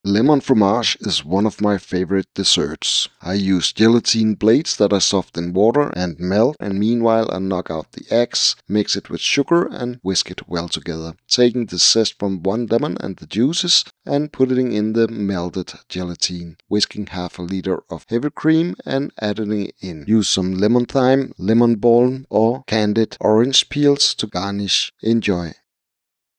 Lemon Fromage
Lemon fromage has always been a favorite dessert of mine. I even eat it on christmas eve as I have been doing the last 40 plus years. Many fellow danes will call it blasphemi because traditionally we eat ris à l'amande which is a rice pudding with whisked heavy cream etc. and I just cant stand it - sorry danes ...

Lemon fromage is on the other hand great upon a dinner. Fresh, light and delicious.

My recipe is simple and as follows for six persons.
8 pcs of gelatineblades
6 eggs
200g sugar
0.5 l heavy cream
zest and juices from one big lemon

Soften the gelatine in water for five minutes and melt with the water that sticks with it. Add in the juices and zest from the lemon. Meanwhile whisk egg and sugar in a bowl and add in the gelatine mix and stir. Also whisk the heavy cream and stir it in after the gelatinemix save a little for garnish. Dont let it set as much as I did in the video.